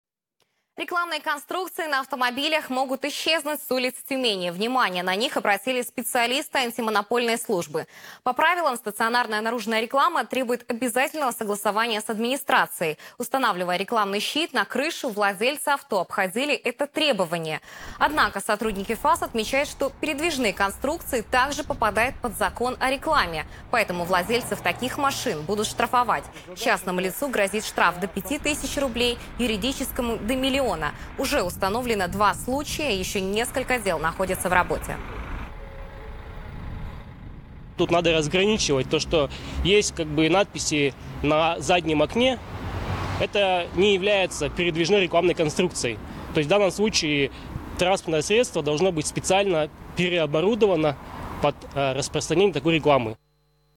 Запрет рекламы на автомобилях
Рекламные конструкции на автомобилях могут исчезнуть с улиц Тюмени. Внимание на них обратили специалисты антимонопольной службы. По правилам стационарная наружная реклама требует обязательного согласования с администрацией. Устанавливая рекламный щит на крышу, владельцы авто обходили это требование. Однако сотрудники ФАС отмечают, что передвижные конструкции также попадают под закон о рекламе. Поэтому владельцев таких машин будут штрафовать. Частному лицу грозит штраф до 5 тысяч рублей, а юридическая фирма может потерять миллион. Уже установлено два случая, ещё несколько дел находятся в работе.
Дмитрий Полухин, заместитель руководителя Управления ФАС по Тюменской области: «Тут надо разграничивать. Есть надписи на заднем окне. Это не является передвижной рекламной конструкцией. В данном случае транспортное средство должно быть специально переоборудовано под распространение такой рекламы».